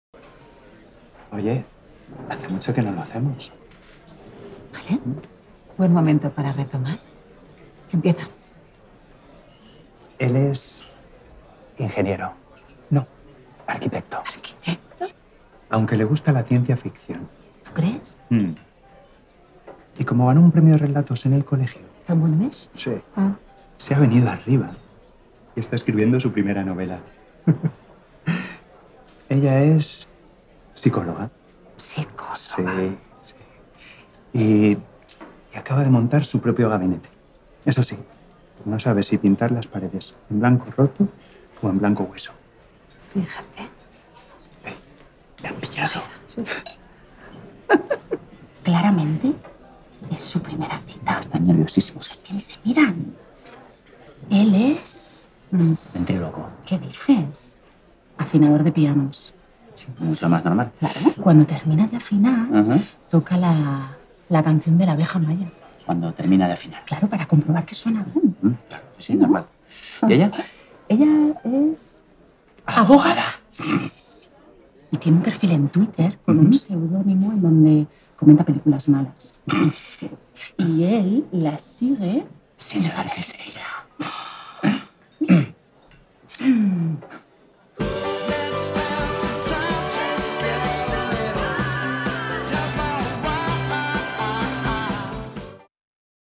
Historias con Vino - El juego
Con una copa de vino se pueden hacer muchas cosas y se pueden recuperar otras poquitas. ¿Tú qué crees que van a hacer estos dos? Dale al play y conoce la historia completa.